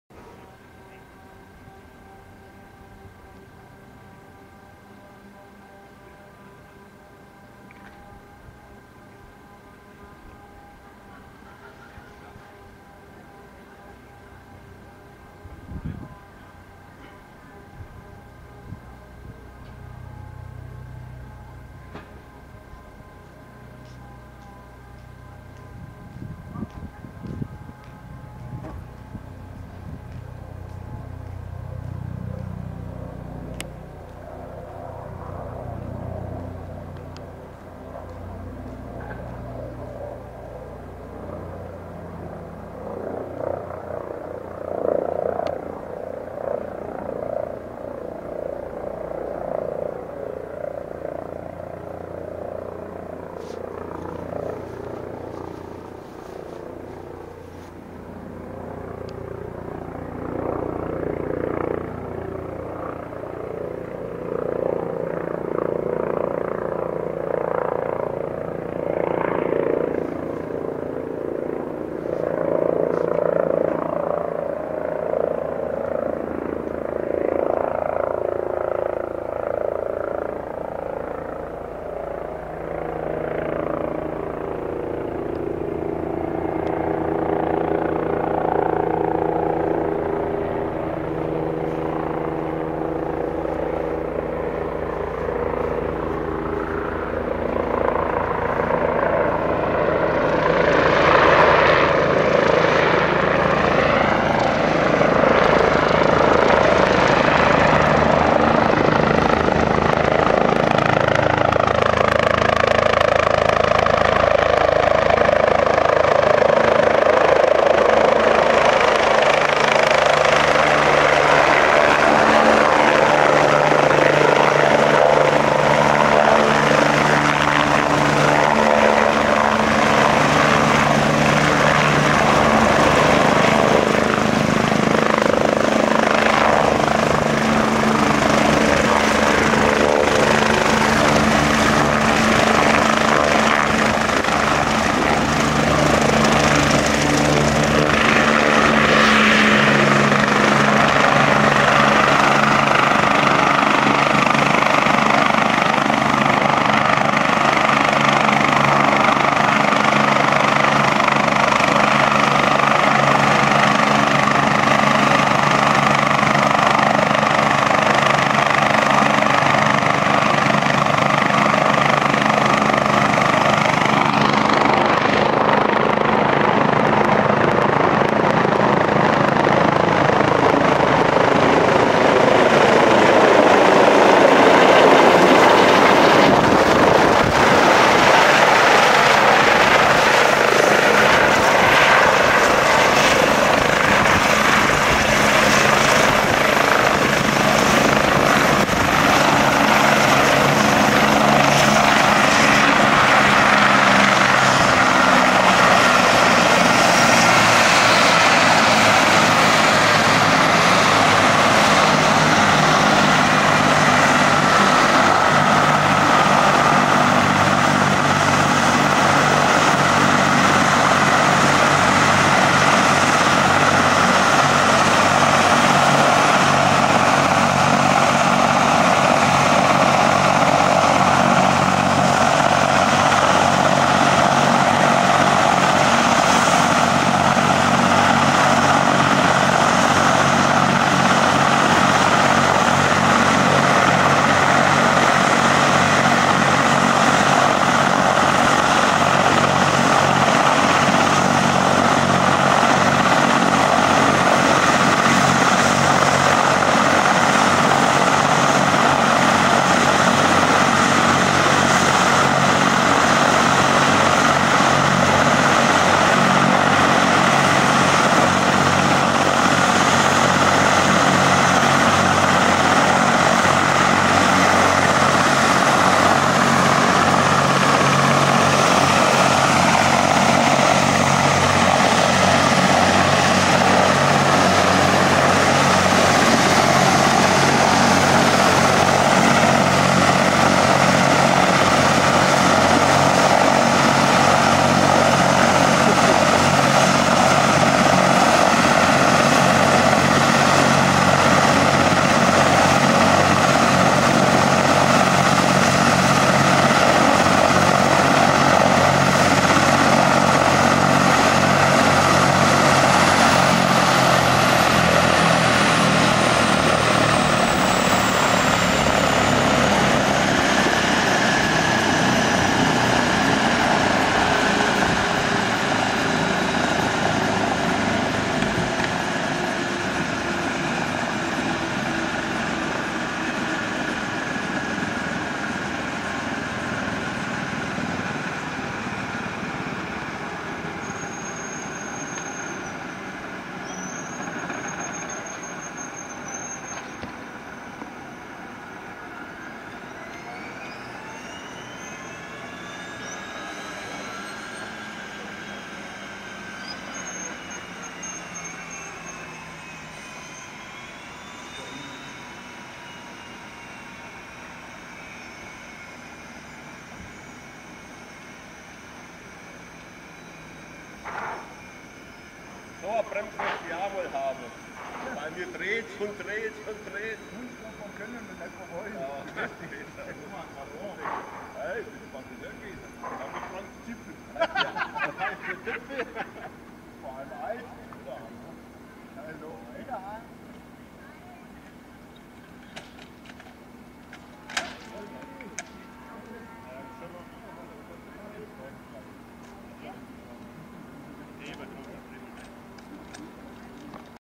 Christoph 51 (Stuttgart) & Christoph 41 (Leonberg) landing in Leonberg
DRF Luftrettung helicopters Christoph 51 (Stuttgart/BK 117 D-HECE) & Christoph 41 (Leonberg/EC 135 P2 D-HDRP) landing in Leonberg